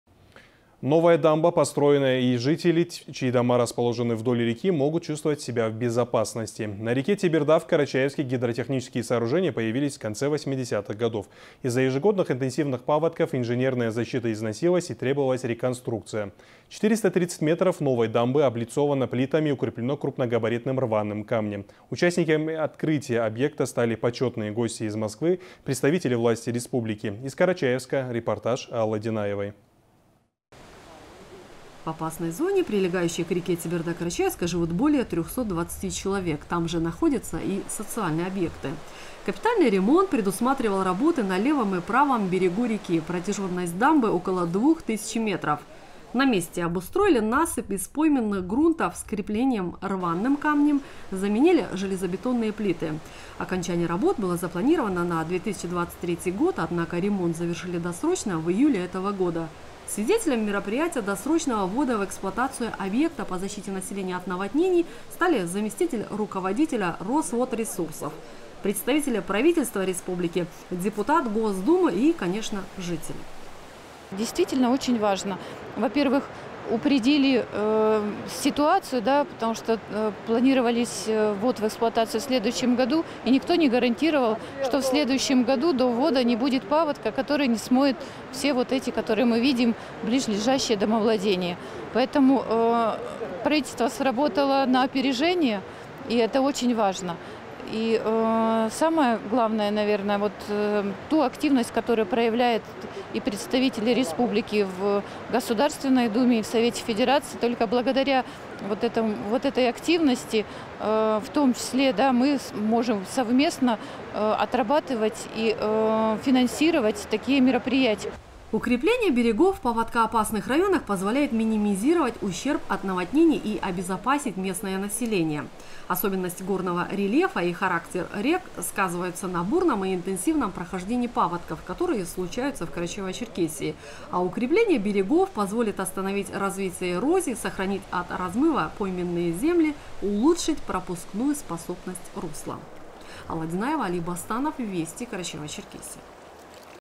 Новая дамба построена и жители могут чувствовать себя в безопасности
Новая дамба построена и жители, чьи дома расположены вдоль реки, могут чувствовать себя в безопасности. На реке Теберда, в Карачаевске, гидротехнические сооружения появились в конце 80-х годов. Из-за ежегодных интенсивных паводков инженерная защита износилась и требовалась реконструкция. 430 метров новой дамбы облицовано плитами и укреплено крупногабаритным рваным камнем